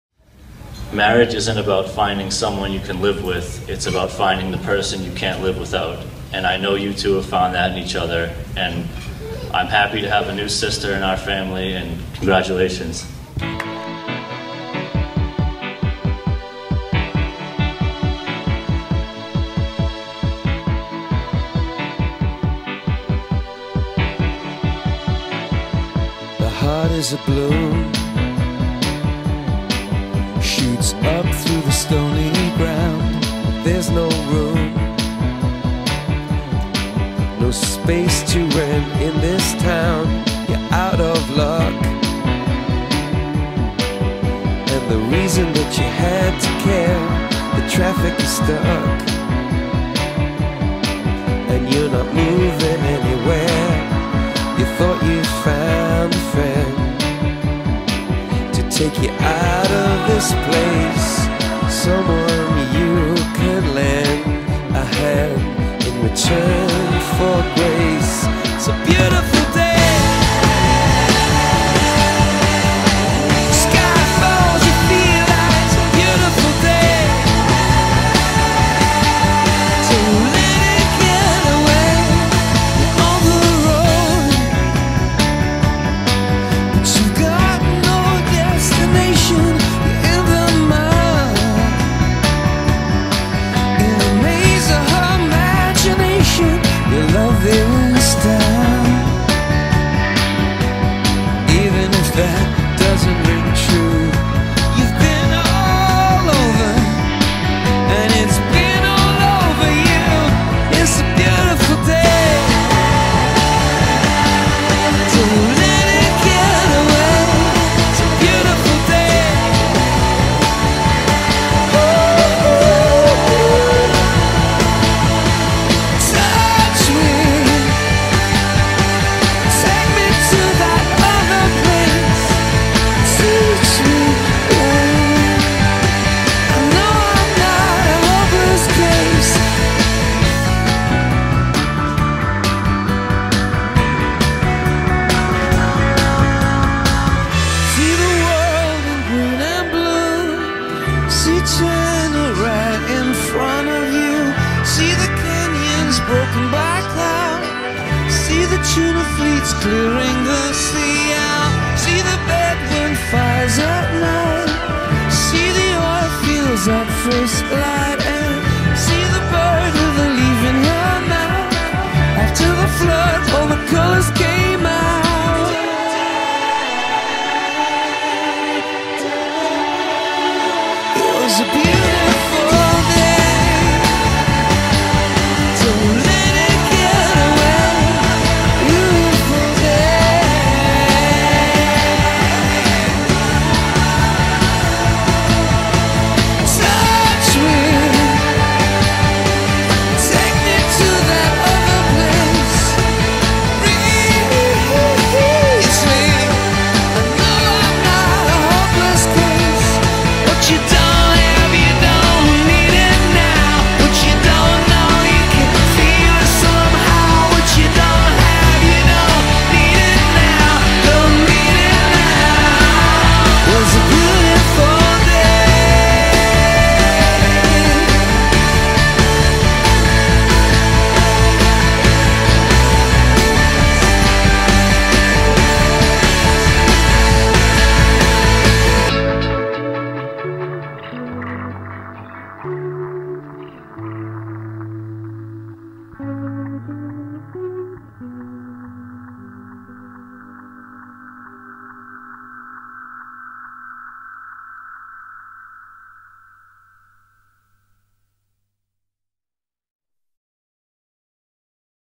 MIchael And Lisa's beautiful Day....
Michael and Lisa's friends and family enjoy their wedding day.